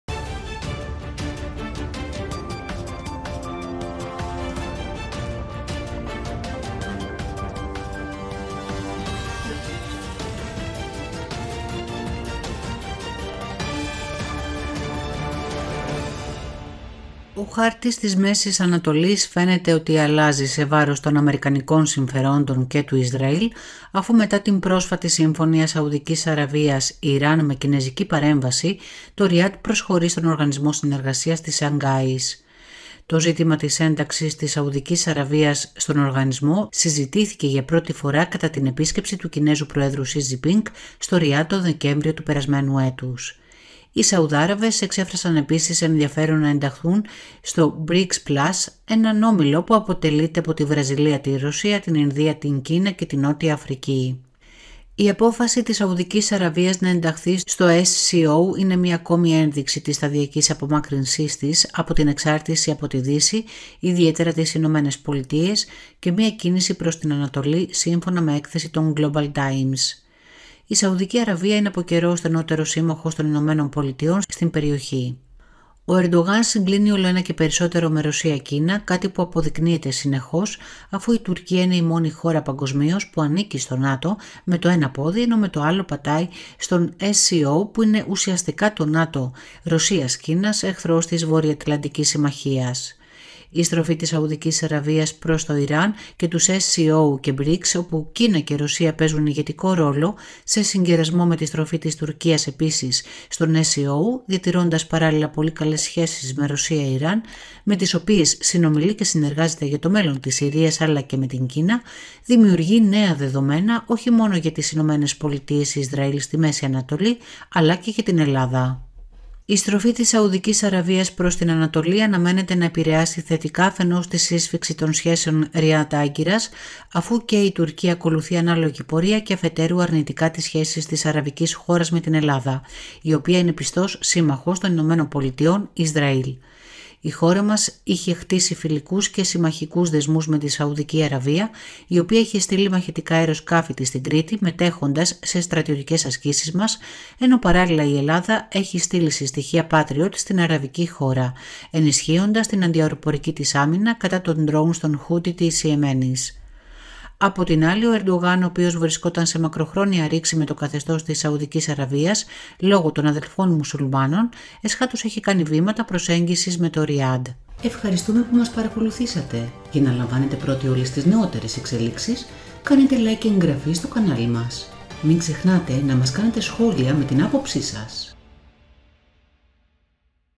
Ο χάρτης της Μ.Ανατολής αλλάζει σε βάρος των ΗΠΑ:Ένταξη Σ.Αραβίας στον Οργανισμό Συνεργασίας Σαγκάης
Ο χάρτης της Μ.Ανατολής φαίνεται ότι αλλάζει σε βάρος των Αμερικανικών συμφερόντων και του Ισραήλ, αφού μετά την πρόσφατη συμφωνία Σαουδικής Αραβίας-Ιράν με Κινεζική παρέμβαση, το Ριάντ προσχωρεί στον Οργανισμό Συνεργασίας της Σαγκάης (SCO).

Το ζήτημα της ένταξης της Σαουδικής Αραβίας στον οργανισμό συζητήθηκε για πρώτη φορά κατά την επίσκεψη του Κινέζου προέδρου Xi Jinping στο Ριάντ τον Δεκέμβριο του περασμένου έτους.

Η στροφή της Σαουδικής Αραβίας προς την Ανατολή, αναμένεται να επηρεάσει θετικά αφενός την σύσφιξη των σχέσεων Ριάντ-Άγκυρας, αφού και η Τουρκία ακολουθεί ανάλογη πορεία και αφετέρου αρνητικά τις σχέσεις της Αραβικής χώρας με την Ελλάδα, η οποία είναι πιστός σύμμαχος των ΗΠΑ-Ισραήλ.

Κεφάλαια:
0:00 - Εισαγωγή
0:18 - Ο χάρτης της Μ. Ανατολής αλλάζει
1:15 - Τουρκία και Σαουδική Αραβία
1:59 - Πως επηρεάζεται η Ελλάδα
2:47 - Αποφώνηση

Η στροφή της Σαουδικής Αραβίας στην Ανατολή
Η απόφαση της Σαουδικής Αραβίας να ενταχθεί στο SCO, μια στρατηγική πρωτοβουλία υπό την ηγεσία της Κίνας και της Ρωσίας, θεωρείται ως ένα ακόμη παράδειγμα της σταδιακής στροφής της προς την Ανατολή.

Η χώρα του Κόλπου επιχειρεί επίσης να ενταχθεί στο BRICS Plus.

Η κυβέρνηση της Σαουδικής Αραβίας αποφάσισε να ενταχθεί στον Οργανισμό Συνεργασίας της Σαγκάης (SCO) ως εταίρος διαλόγου την Τετάρτη 29 Μαρτίου, καθιστώντας  την, ως την  τρίτη αραβική χώρα που το πράττει μετά την Αίγυπτο και το Κατάρ.

Η απόφαση για ένταξη στην SCO λήφθηκε σε συνεδρίαση του υπουργικού συμβουλίου με επικεφαλής τον Βασιλιά Σαλμάν μπιν Αμπντουλαζίζ Αλ Σαούντ, σύμφωνα με το κρατικό πρακτορείο Τύπου της Σαουδικής Αραβίας (SPA).

Το καθεστώς εταίρου διαλόγου της Σαουδικής Αραβίας, μπορεί να μετατραπεί σε πλήρη μέλος της ομάδας στο μέλλον.

Οι Σαουδάραβες εξέφρασαν επίσης ενδιαφέρον να ενταχθούν στο BRICS Plus, έναν όμιλο που αποτελείται από τη Βραζιλία, τη Ρωσία, την Ινδία, την Κίνα και τη Νότια Αφρική.

Η απόφαση της Σαουδικής Αραβίας να ενταχθεί στο SCO είναι μια ακόμη ένδειξη της σταδιακής απομάκρυνσής της από την εξάρτηση από τη Δύση, ιδιαίτερα τις ΗΠΑ, και μια κίνηση προς την Ανατολή, σύμφωνα με έκθεση των Global Times.

Η Σαουδική Αραβία είναι από καιρό ο στενότερος σύμμαχος των ΗΠΑ στην περιοχή.

Οι σχέσεις Ριάντ-Τεχεράνης-Πεκίνου

Η κίνηση ακολουθεί μια συμφωνία προσέγγισης που υπογράφηκε μεταξύ της Σαουδικής Αραβίας και του Ιράν, με τη διευκόλυνση των Κινέζων.

Θεωρούμενοι αντίπαλοι για την περιφερειακή ηγεμονία πριν από τη συμφωνία, η Σαουδική Αραβία και το Ιράν συμφώνησαν τώρα να αποκαταστήσουν τις διπλωματικές σχέσεις μετά από επτά χρόνια. Και οι δύο χώρες συμφώνησαν επίσης να μην παρέμβουν στις εσωτερικές υποθέσεις της άλλης.

Η συμφωνία προβλέπει επίσης τη δημιουργία μεγαλύτερης πολιτικής και στρατηγικής συνεργασίας μεταξύ των δύο εθνών στο μέλλον.

Οι Global Times ανέφεραν ότι η Σαουδική Αραβία έχει επίσης λάβει περαιτέρω μέτρα για να εδραιώσει τους δεσμούς της με την Κίνα.

Η Saudi Aramco υπέγραψε πρόσφατα συμφωνία με κινεζικές εταιρείες για τη δημιουργία κοινού πετροχημικού έργου στο Panjin στην επαρχία Liaoning της Κίνας. Η συμφωνία ανέρχεται σε περισσότερα από 12 δισεκατομμύρια δολάρια.

SCO και "Οργανισμός Τουρκικών κρατών"

Η SCO ιδρύθηκε το 2001 με μέλη την Κίνα, το Καζακστάν, το Κιργιστάν, τη Ρωσία, το Τατζικιστάν και το Ουζμπεκιστάν. Είναι ο διάδοχος του ομίλου Shanghai Five. Αργότερα, η Ινδία και το Πακιστάν έγιναν επίσης πλήρη μέλη του πολιτικού και στρατηγικού ομίλου, και το Ιράν πρόκειται να ενταχθεί μέχρι τα τέλη του τρέχοντος έτους.

 Μέλη παρατηρητές του οργανισμού είναι  Λευκορωσία, Μογγολία και Αφγανιστάν, ενώ συνομιλητές  είναι η Τουρκία, Αρμενία, Αζερμπαϊτζάν, Καμπότζη, Νεπάλ, Σρι Λάνκα και εσχάτως Αίγυπτος και Κατάρ

Ο "Οργανισμός Τουρκικών κρατών"  έχει ως 5 μόνιμα κράτη-μέλη που είναι η Τουρκία, το Καζακστάν, το Αζερμπαϊτζάν, το Κιργιστάν και το Ουζμπεκιστάν, ενώ παρατηρητές είναι  το Τουρκμενιστάν και η Ουγγαρία